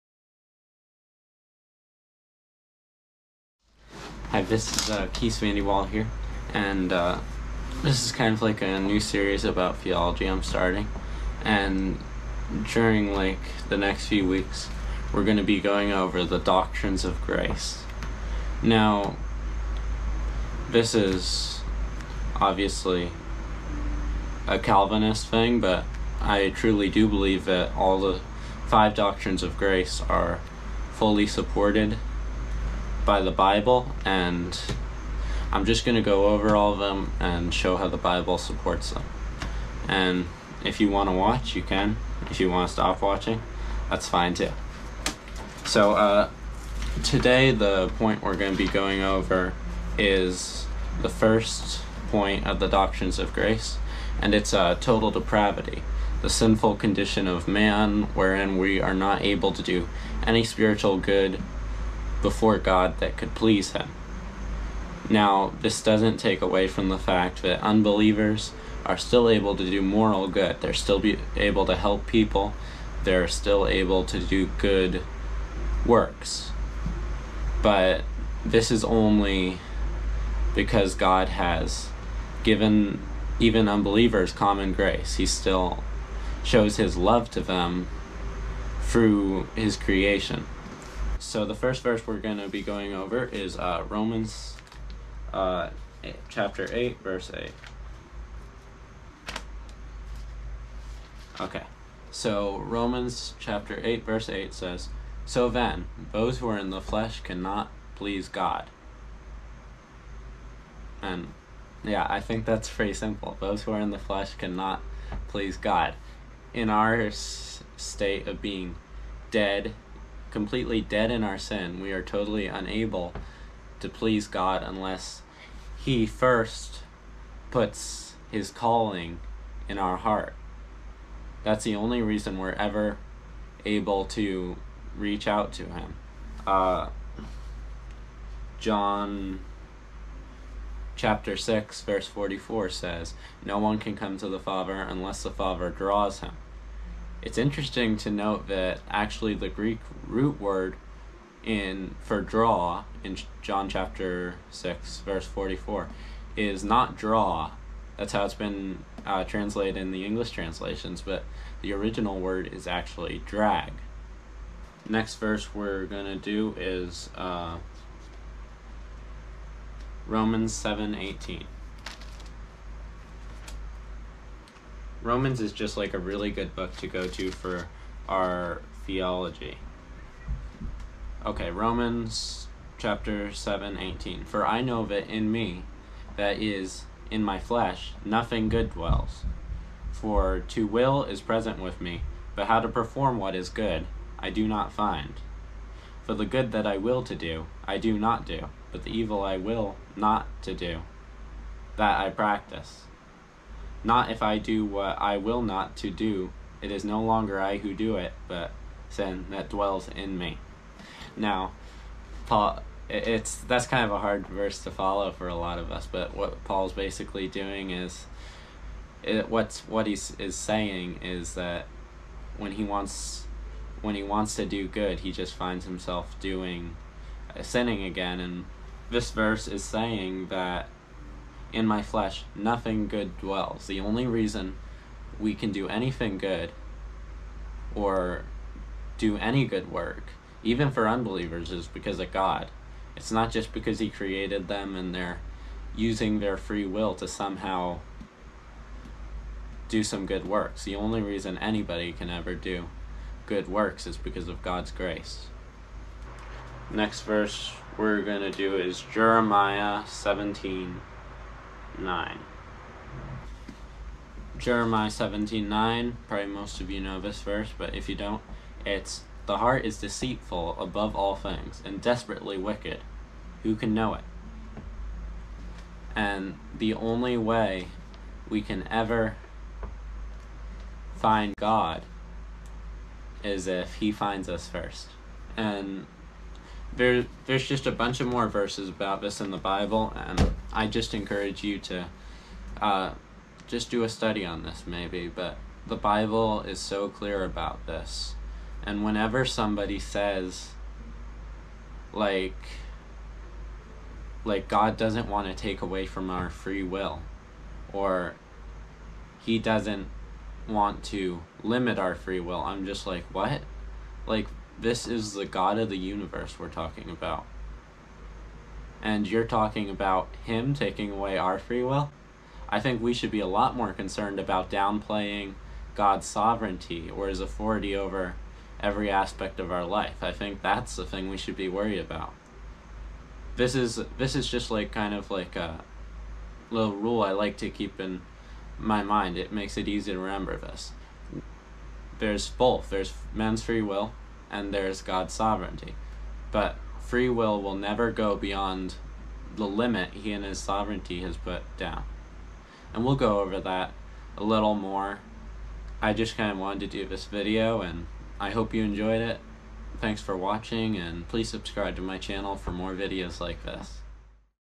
The Doctrines of Grace - Total Depravity
This new series will be going over the Doctrines of Grace and seeing how they line up with Scripture